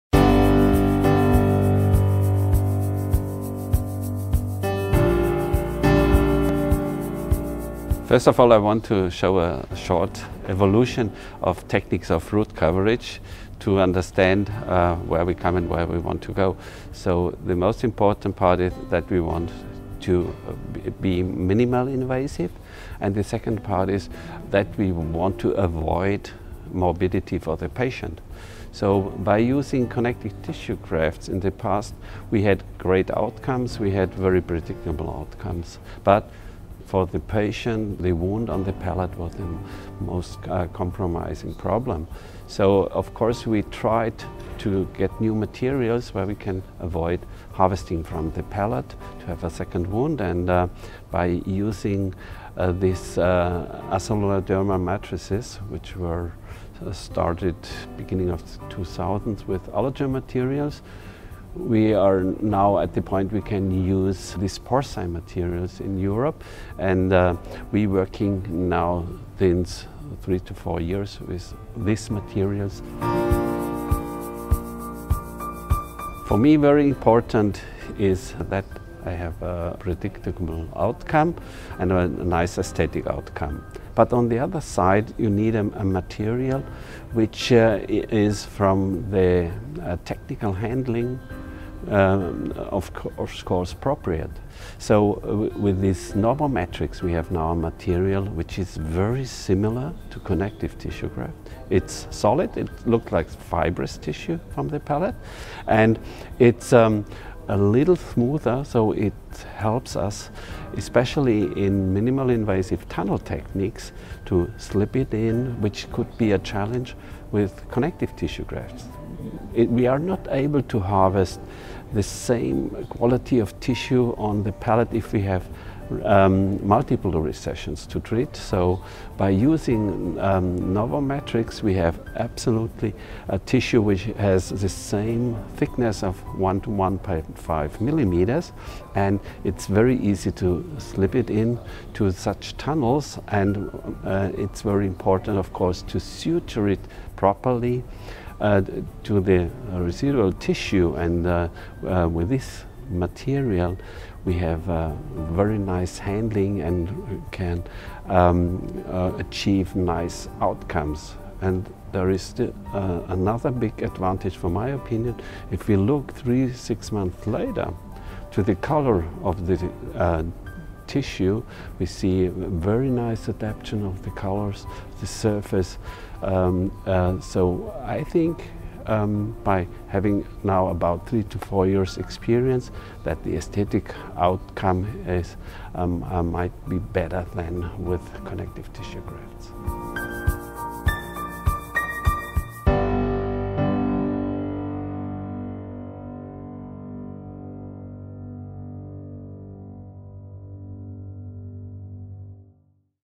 Innovative treatment concepts – how to improve efficiency and regeneration
As a part of the Corporate Forum at the International Osteology Symposium 2019 in Barcelona, PD Dr. Gerhard Iglhaut (Germany) presented innovative treatment concepts for BioHorizons Camlog. In the video he reports about the new xenograft NovoMatrix.